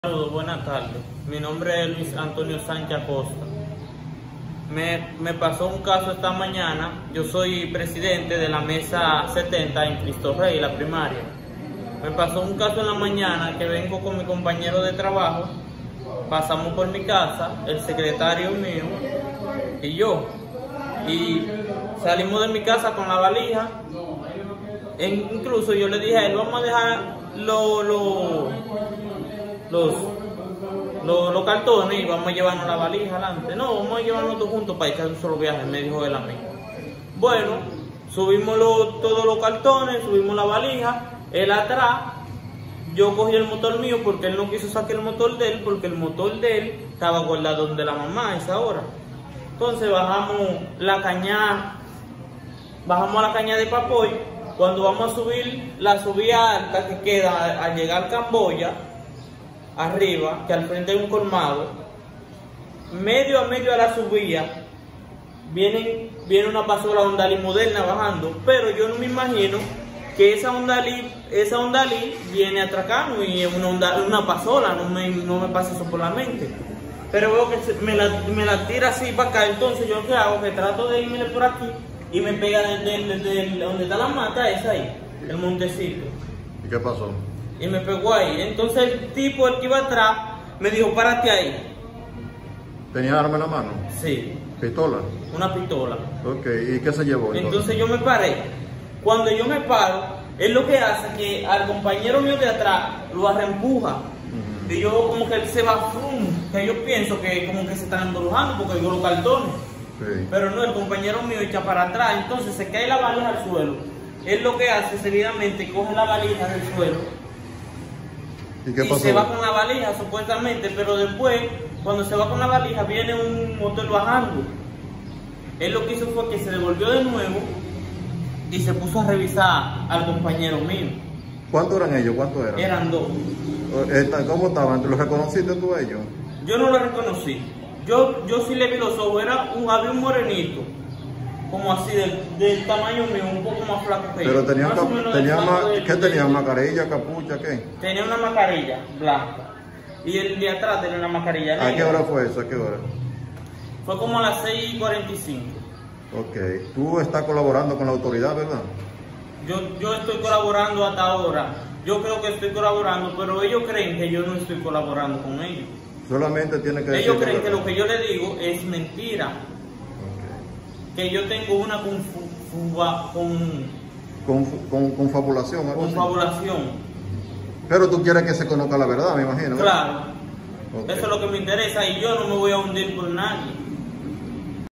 Robo de valija en Barahona. JCE.
Valija de la mesa 0070, Cristo Rey, Barahona. Cómo ocurrió según el presidente de la mesa.